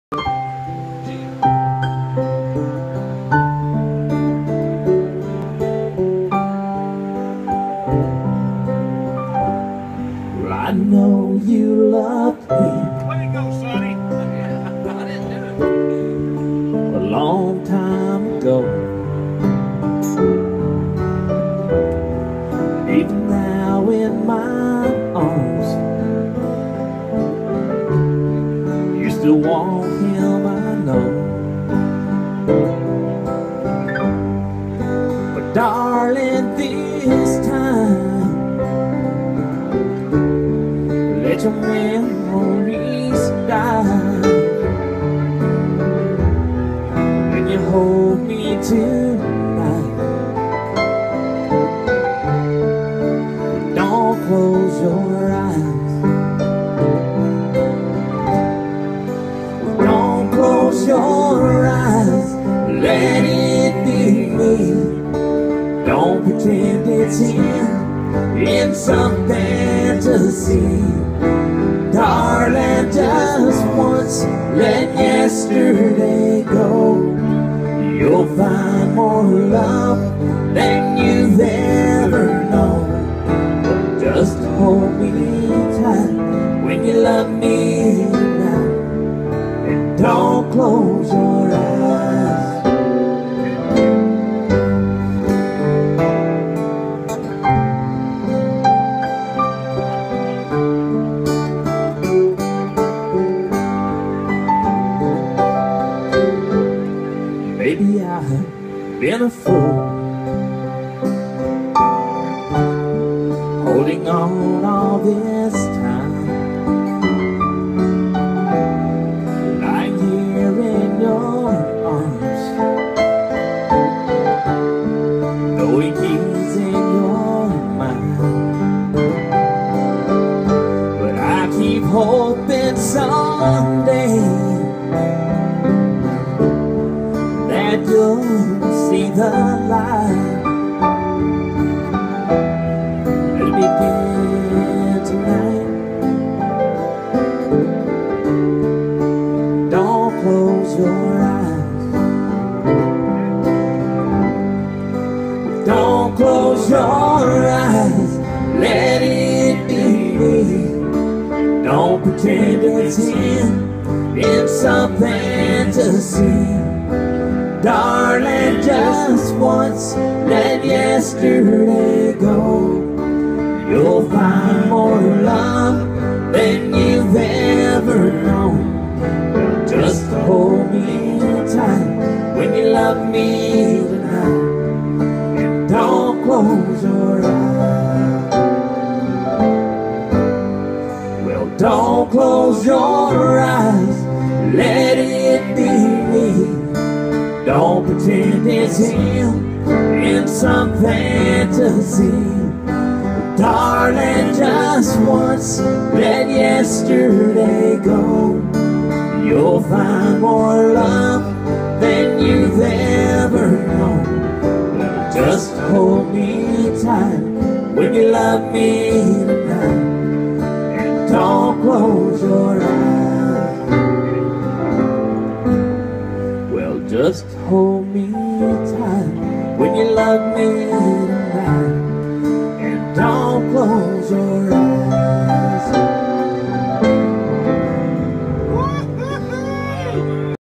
Gary R. Key's Birthday Bash. Willie Poe Sings.
Willie Poe sings with Phil Austin, Sonny Skinner and Eric Swann at Gary R. Key's birthday bash.